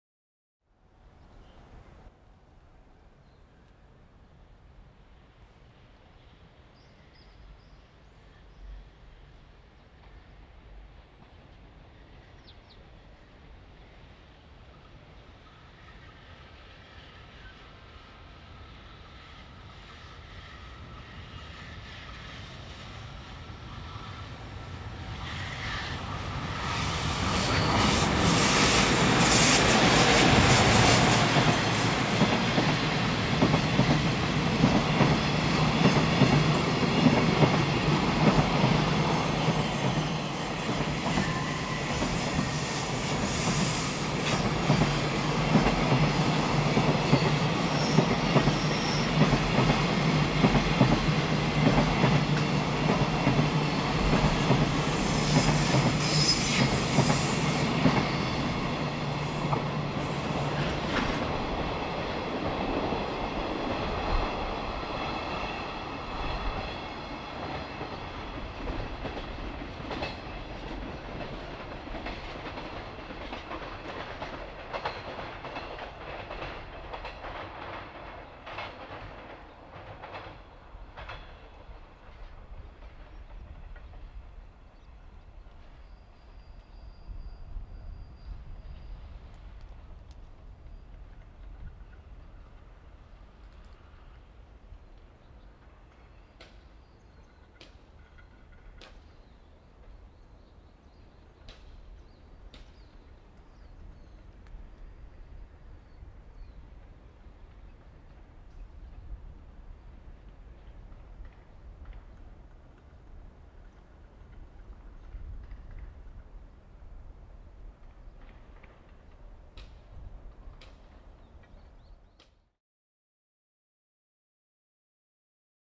JR貨物 EF200-2号機+コキ22B 貨物列車1091レ 玖波駅通過 2013.6
貨物列車1091レです。牽引機はEF200-2号機(吹田機関区所属)でした。1号機が廃車になったので試作機の901号機を除いて量産型の中では最若番です。
2013年6月撮影